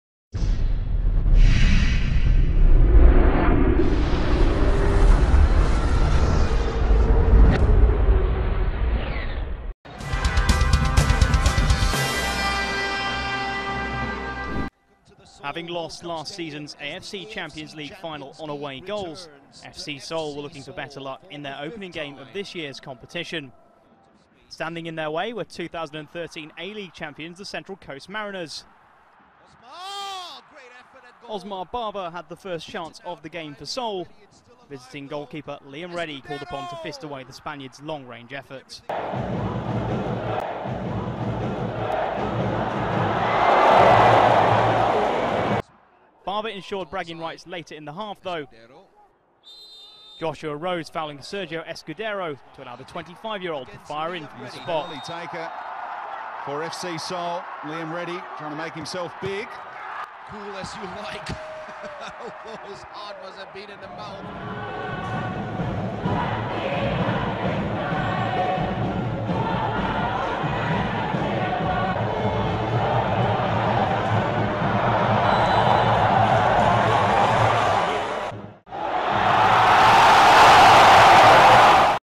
Osmar Barba Ibanez - FC Seoul V Central Coast Mariners - AFC Champions League 2014
Osmar Barba Ibanez scores on debut for FC Seoul in their 2-0 win over Central Coast Mariners in the 2014 AFC Champions League at the Seoul World Cup Stadium.